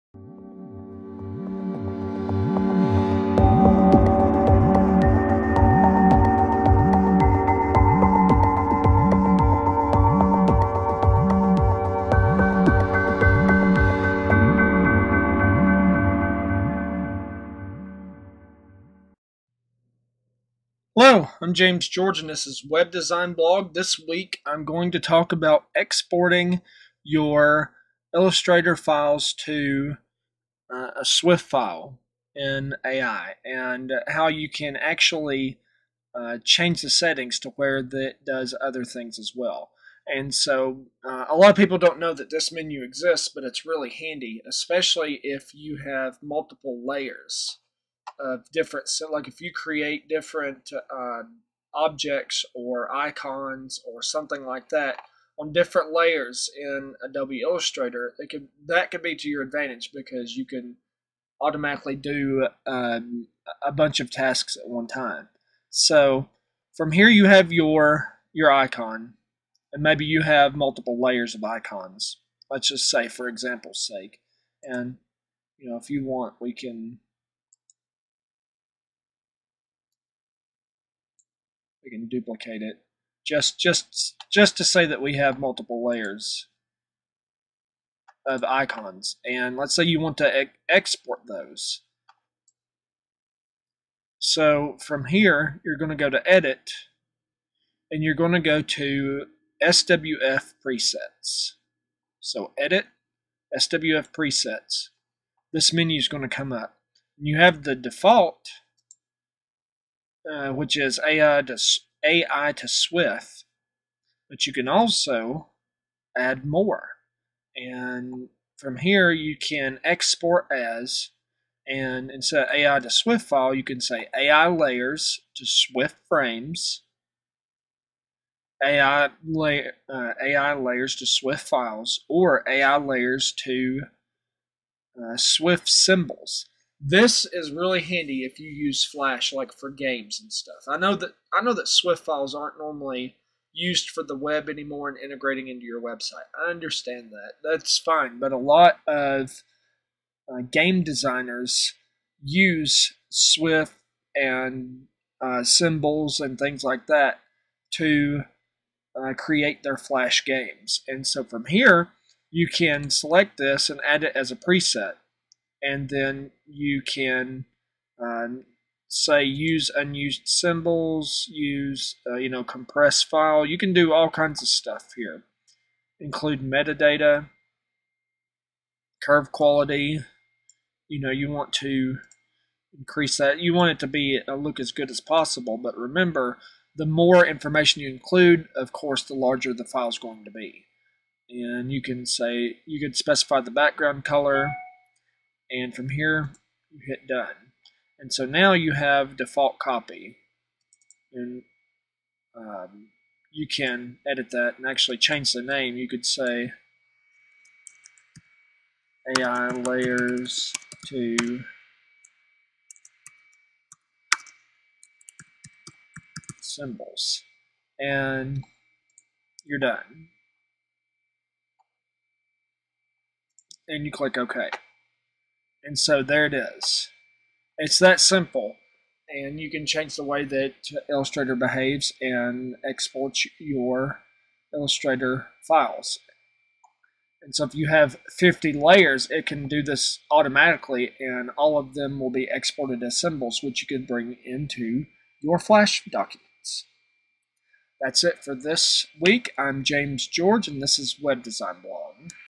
Export Illustrator Layers to Flash Symbols
When you're running your own business, productivity is one of the biggest keys to your success. Any time that you can increase productivity in your workflow on a day-to-day basis you're going going to decrease your costs and increase your profits. To put it into perspective, if you saved just 2% in productivity you would save one week out of the year. That is an incredible amount of savings, especially when you are considering your own work time, or if you have a team of workers working for you. Knowing tricks with your software to save time is a great way to increase productivity. Adobe Illustrator is a great tool for design productivity. You can export layers and files to different formats, which will save you a ton of time. You won't have to go to file and then "save as" for each individual layer or file. In this video, I'm going to show you how to export Illustrator lawyers to flash symbols instantly.